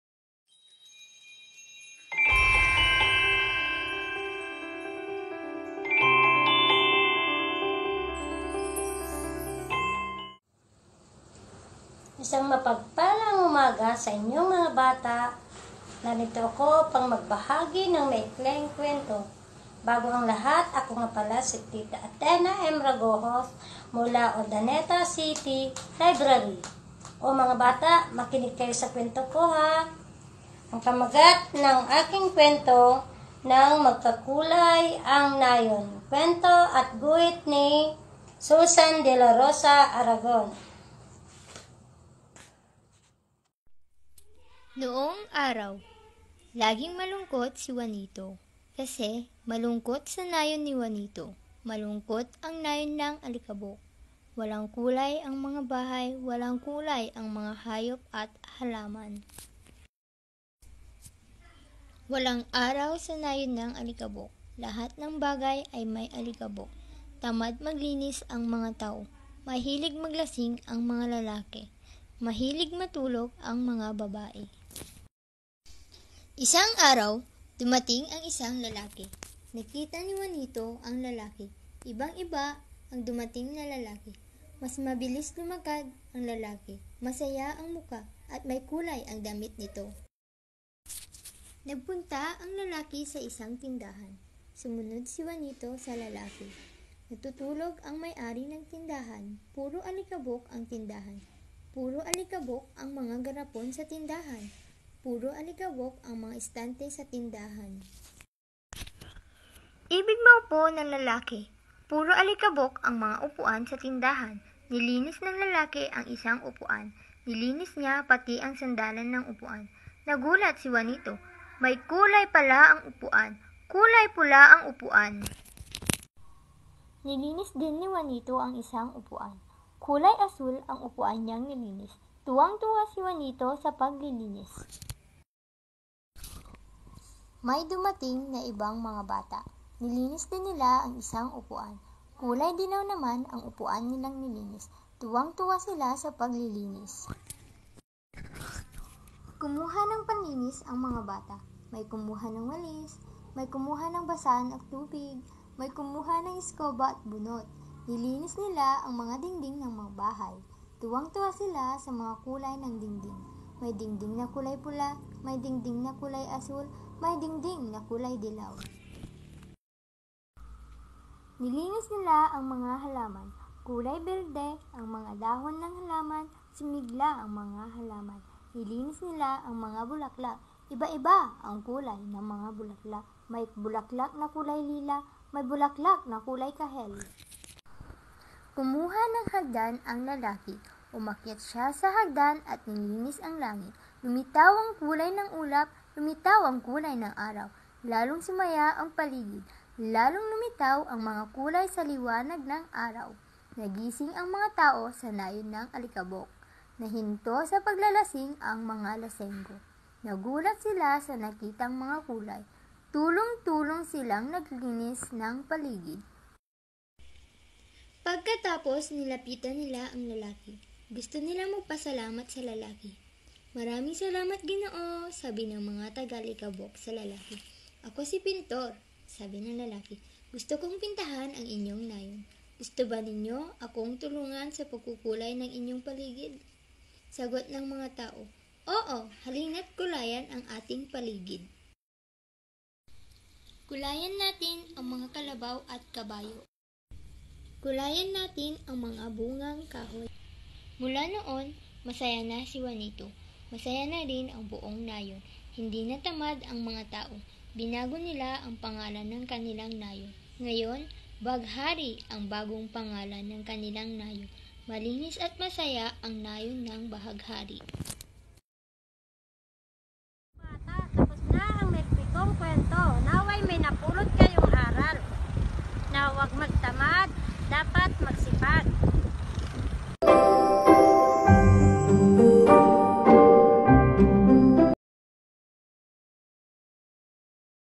"Nang Magkakulay ang Nayon"
Mga bata ating tunghayan ang ating Kwentistang si
Athena Ragojos para sa kanyang kwentong pinamagatang
"Nang Magkakulay ang Nayon" Kwento at guhit ni Susan Dela Rosa Aragon